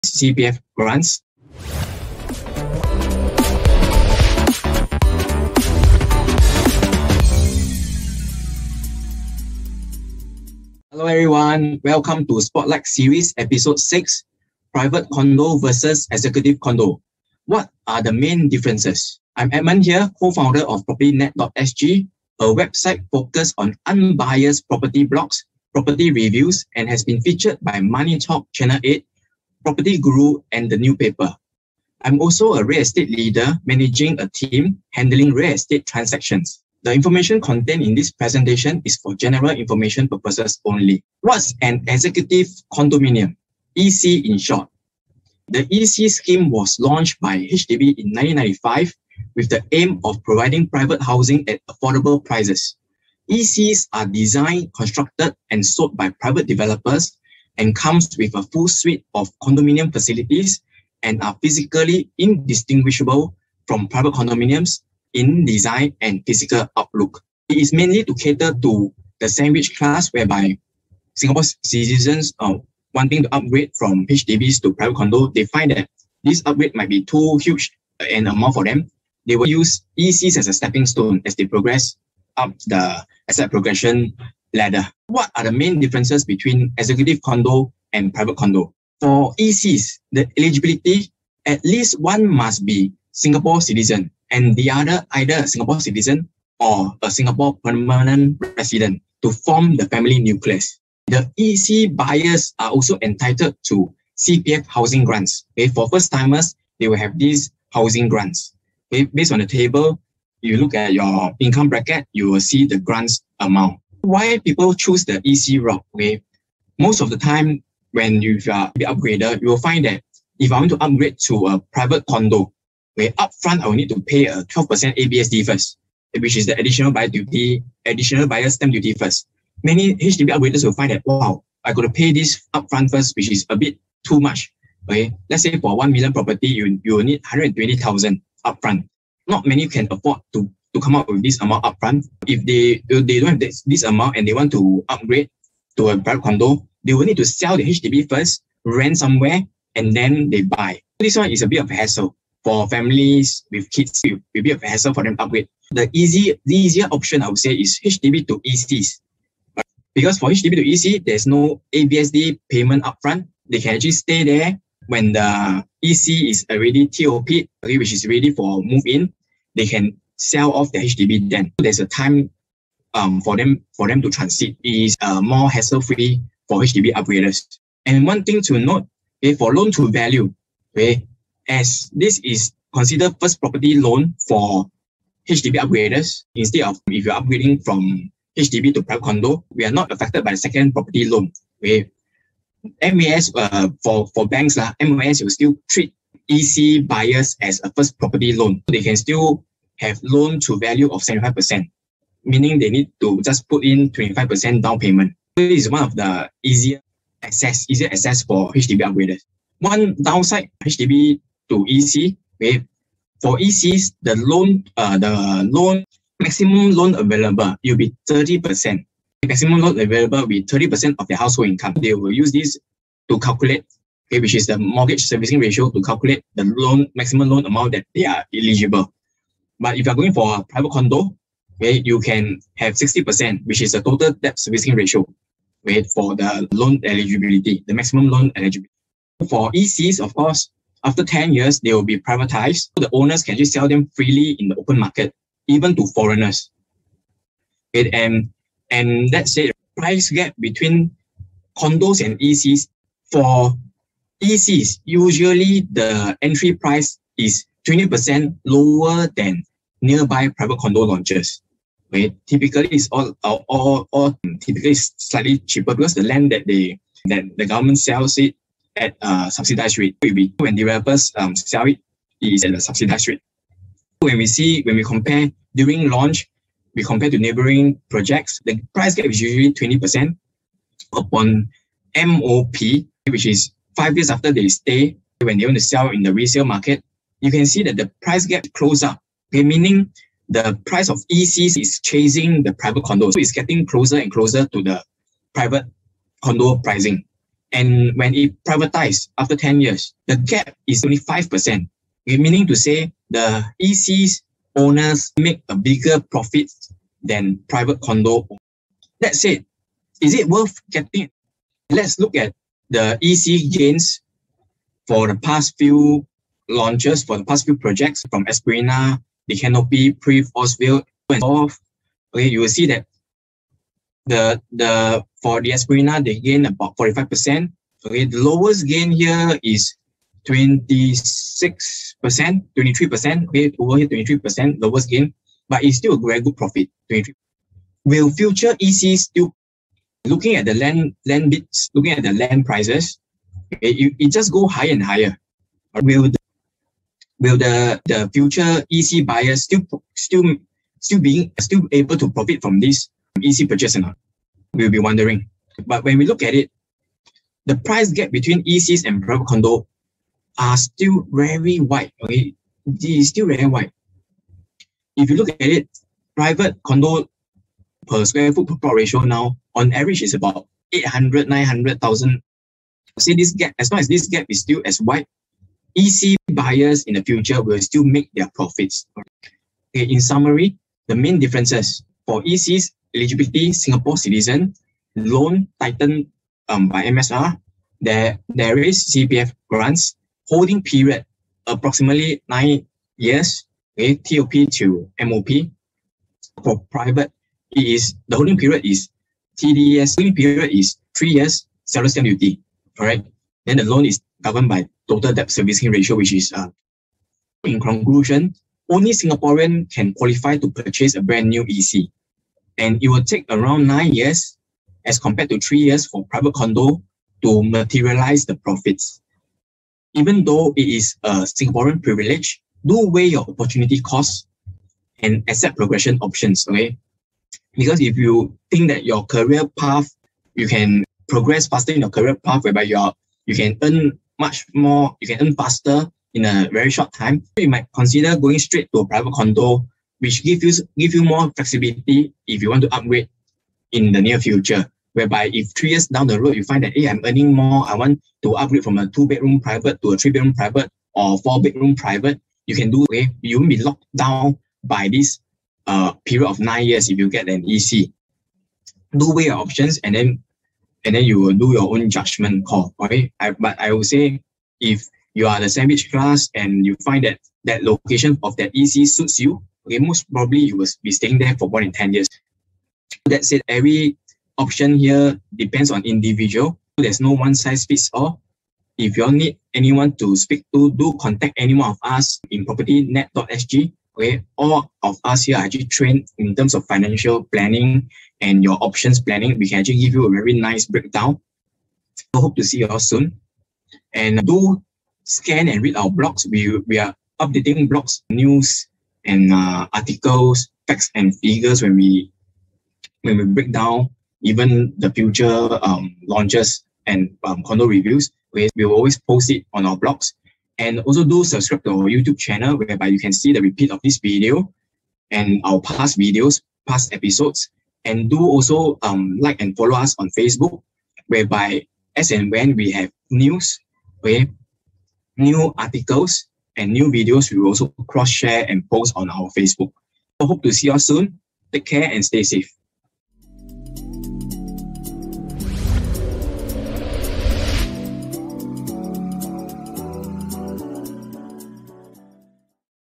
Spotlight Series Ep.6 - Executive Condo Vs Private Condo. What are the main differences?
Presented By: Co-Founder PropertyNet.SG Edmund Ee